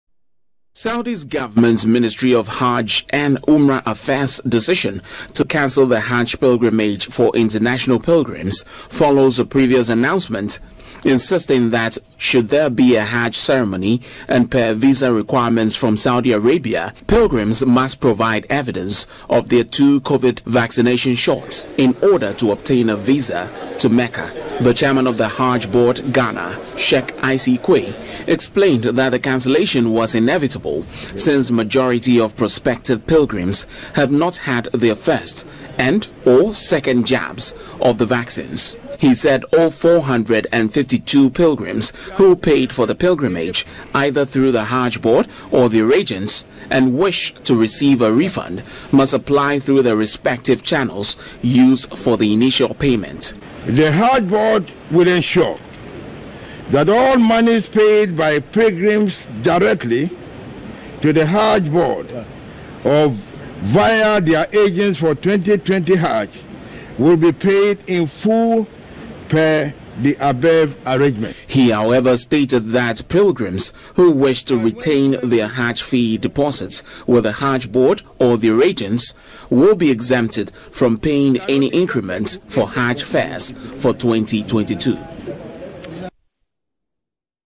Ghana cancels Hajj amid concerns over COVID
The Ghana Hajj Board says there will be no pilgrimage to Mecca this year.